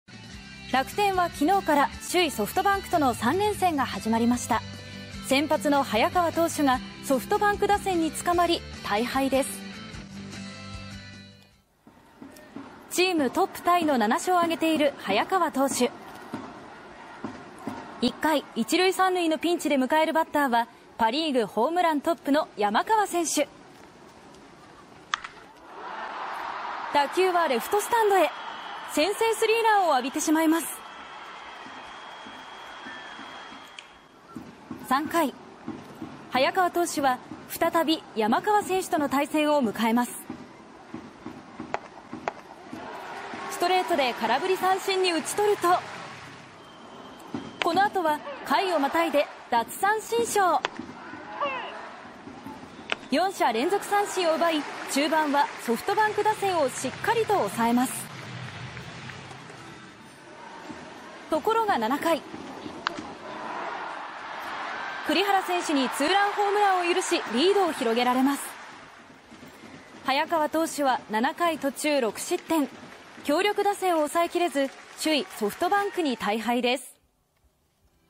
楽天イーグルス 投手陣がソフトバンク強力打線を抑えきれず大敗 ３０日
楽天イーグルスは３０日から首位ソフトバンクとの３連戦です。初戦は先発の早川がソフトバンク打線につかまり大敗です。